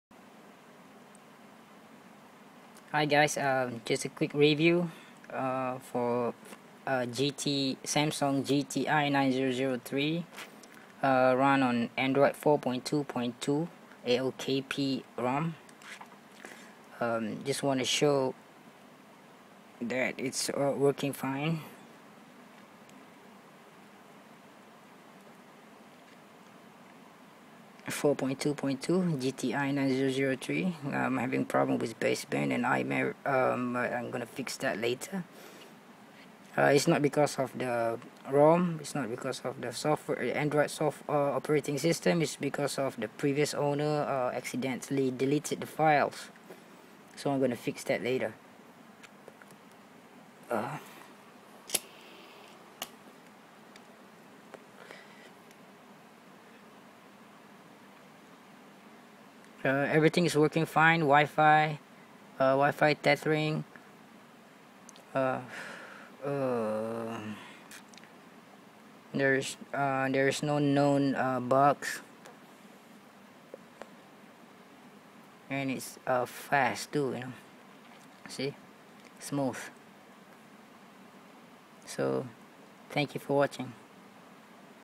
Android 4.2.2 installed on Gt-i9003
Working android 4.2.2 on Samsung gt-i9003. For those who wanna how to install it, I will upload the video later.